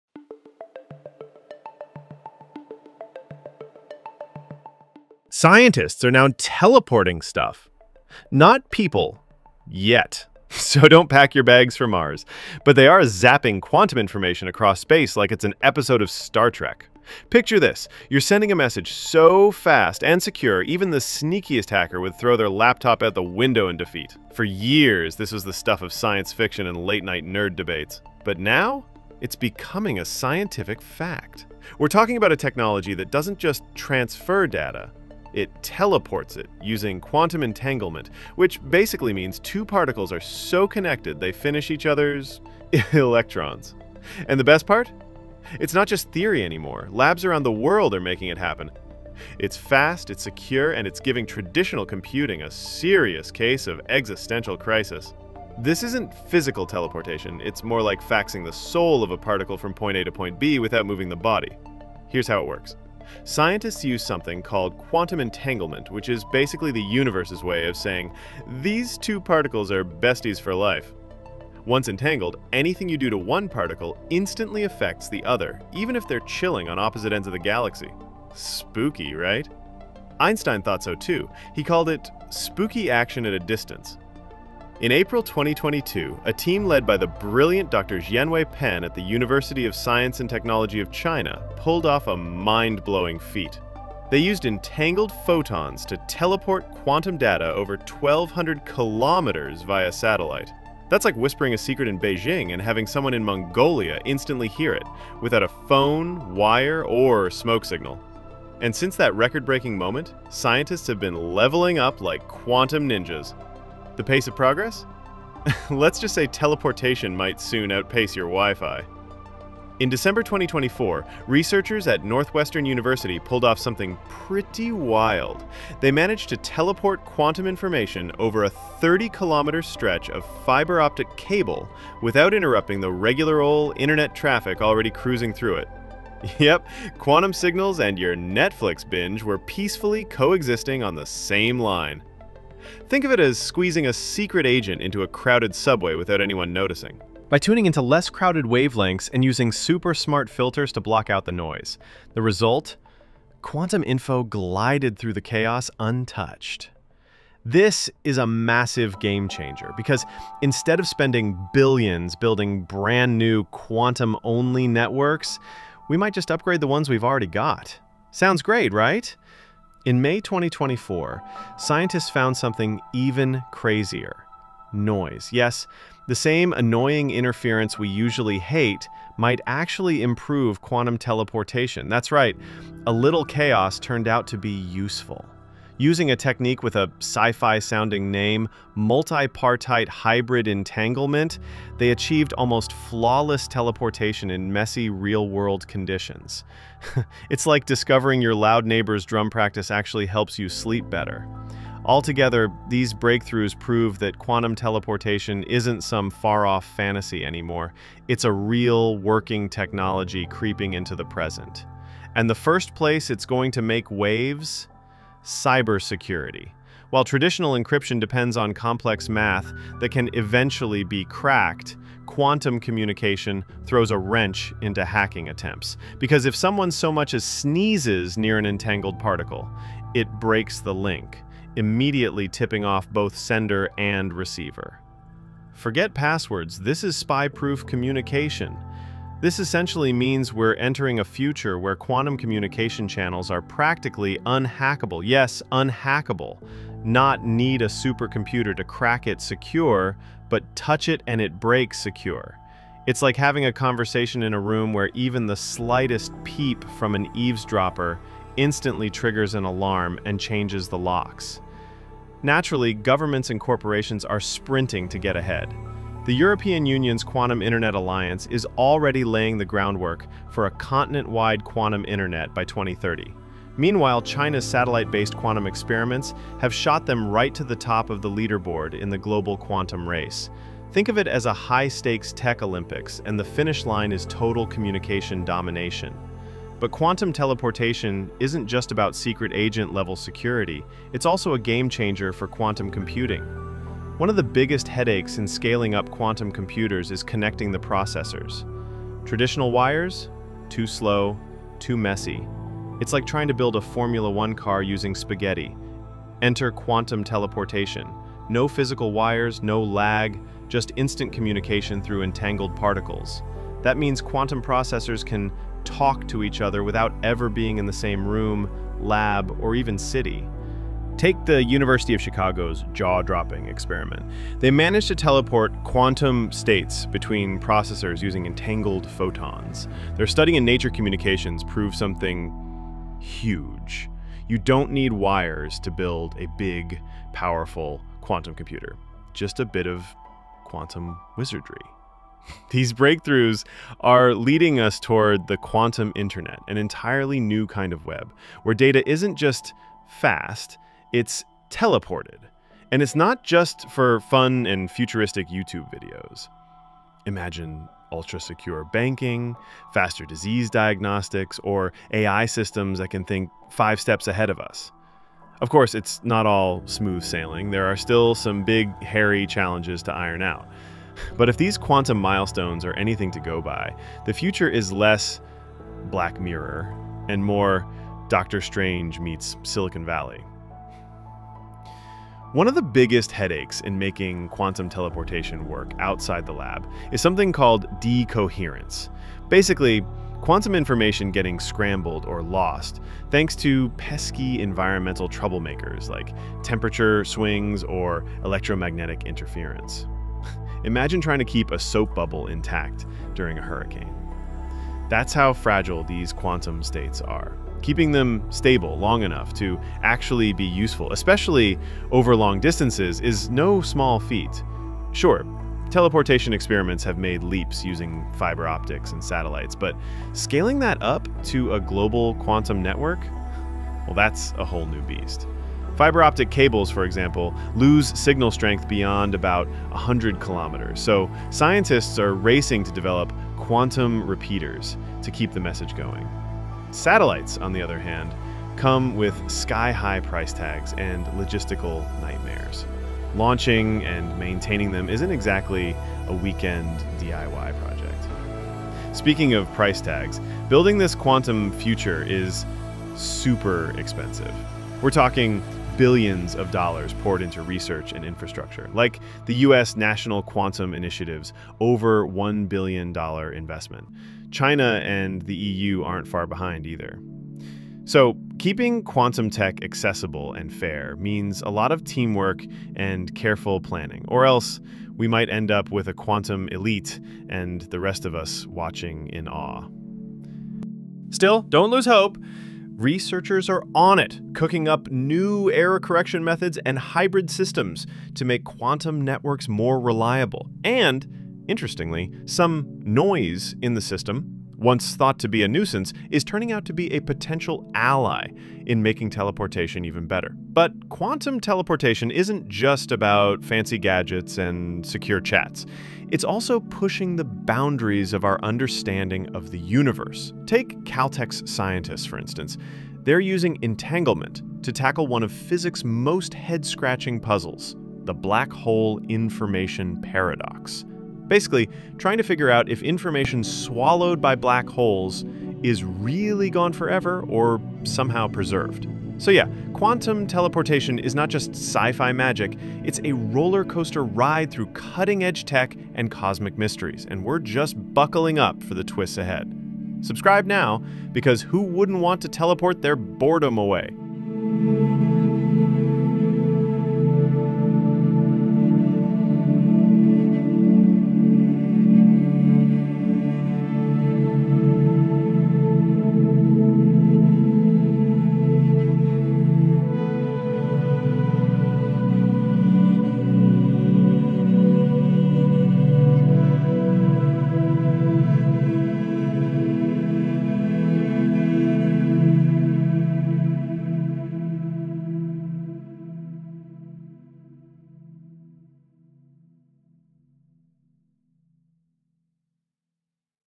Quantum Teleportation Made Possible! Scientists Achieved Near-Perfect Results
Quantum teleportation made possible! In a groundbreaking experiment, scientists have successfully transferred quantum information with near-perfect accuracy—an astonishing 99% success rate. This jaw-dropping achievement is more than just sci-fi come to life; it’s a peek into the future of ultra-secure communication and quantum computing.

By harnessing the bizarre principles of quantum entanglement, researchers managed to teleport quantum states between particles over long distances without physically moving the particles themselves. This isn’t about teleporting people yet—but it's laying the foundation for revolutionary tech like quantum internet, where information can move instantly and securely across the globe.
Dive into the science behind this mind-bending breakthrough, how it works, and what it could mean for the future of technology and physics. If you love quantum mechanics, tech revolutions, or just having your mind blown—this one's for you.